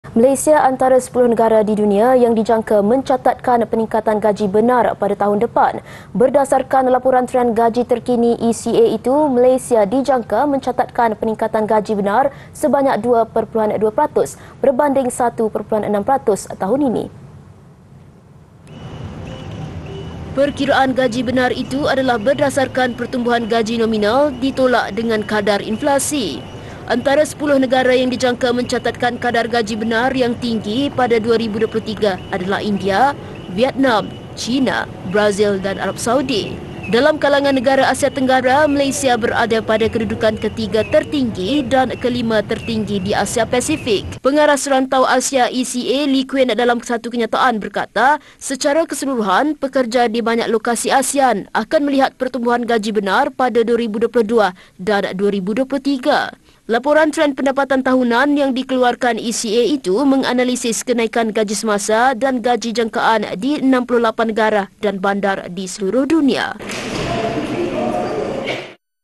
Pekerja Malaysia Naik Gaji 2.2 Peratus Tahun Depan
Malaysia antara 10 negara di dunia yang dijangka mencatatkan peningkatan gaji benar pada tahun depan.

Berdasarkan Laporan Trend Gaji terkini ECA itu, Malaysia dijangka mencatatkan peningkatan gaji benar sebanyak 2.2 peratus berbanding 1.6 peratus tahun ini.